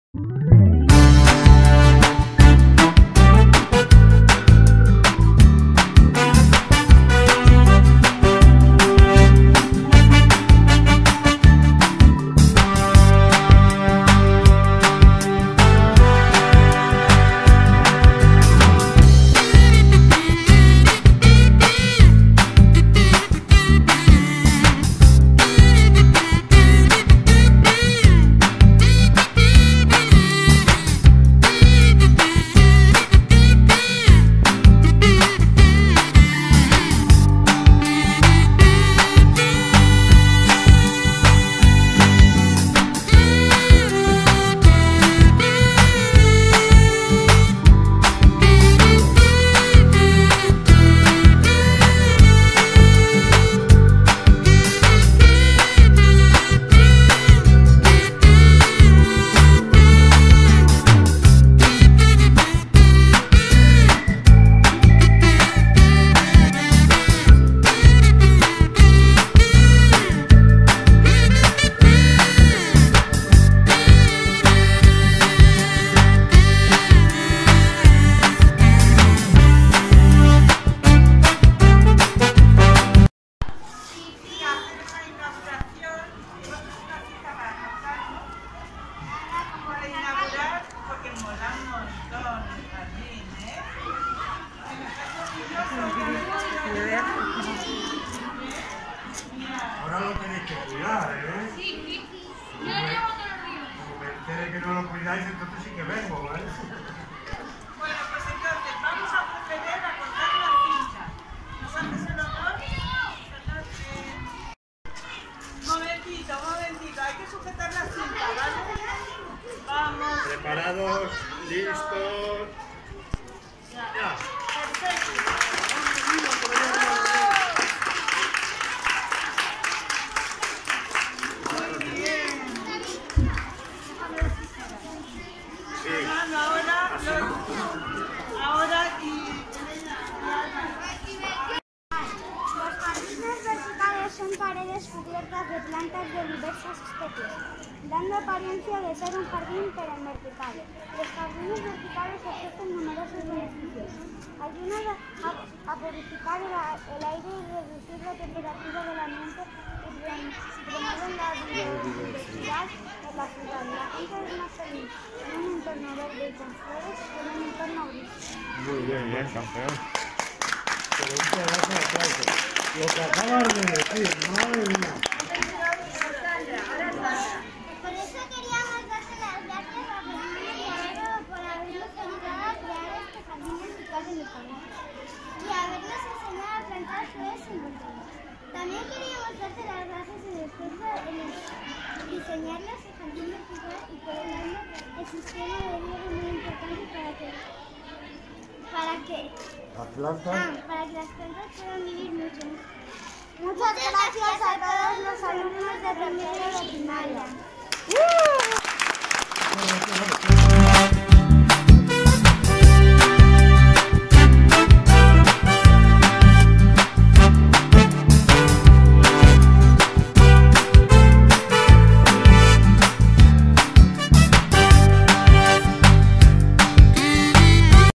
JARDIN VERTICAL COLEGIO LEOPOLDO CALVO SOTELO
Jardín vertical realizado con alumnos y profesoras de 3º de primaria, como ejercicio de Educación ambiental